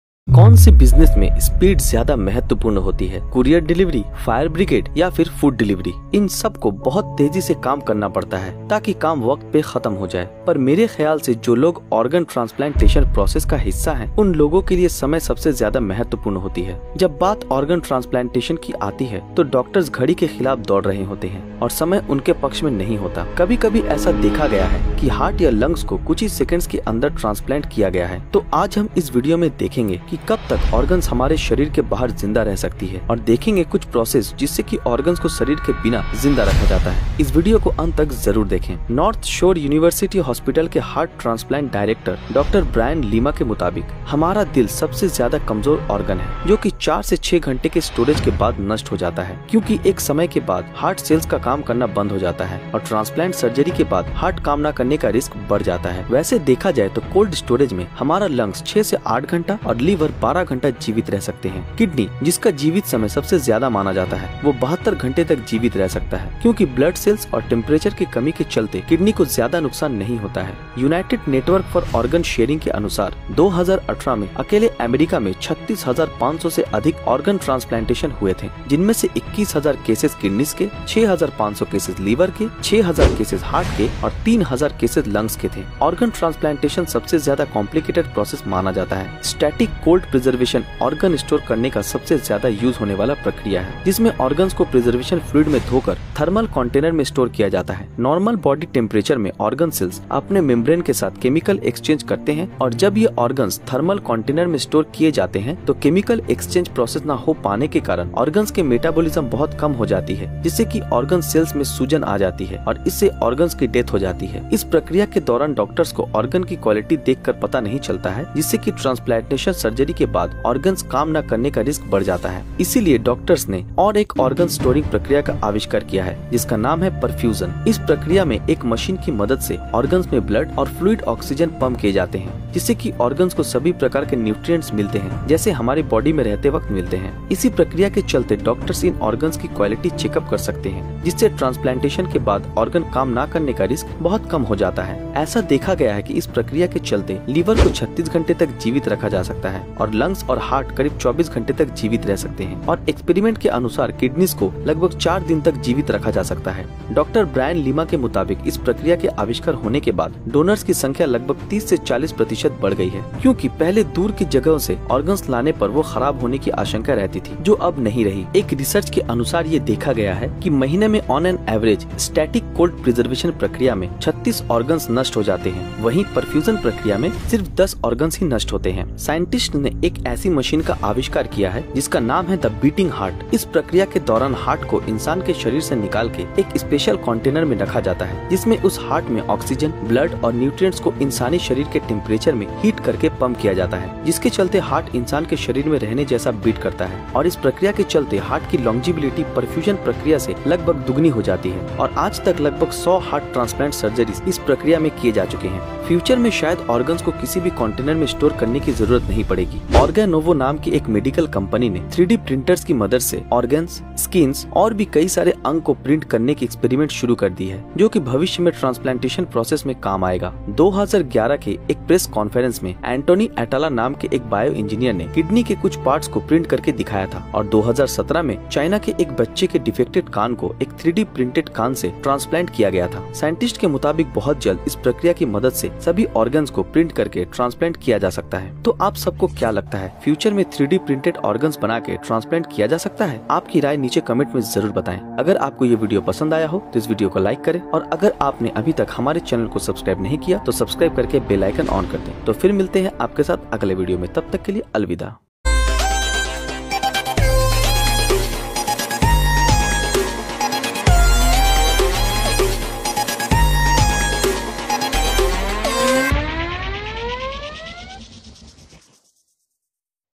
इंसान के अंग शरीर के बाहर कितने समय तक जिंदा रहते हैं | How Long Organs Alive Outside Your Body
Typical storage times are 30 hours or less for a
kidney, less than 12 hours for a pancreas or liver, t
and less than 6 hours for a heart or lungs. These
times vary because of the relative speed at which
educational or personal use tips the balance in favourite